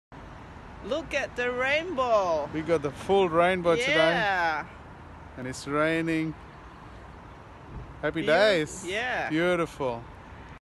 Rainbow at Sculpture PT - a little reminder that everyone is welcome at Sculpture !
We voted yes!

Sculpture PT would like to encourage EVERYONE to test our inclusiveness and try a complimentary session with us - obligation free.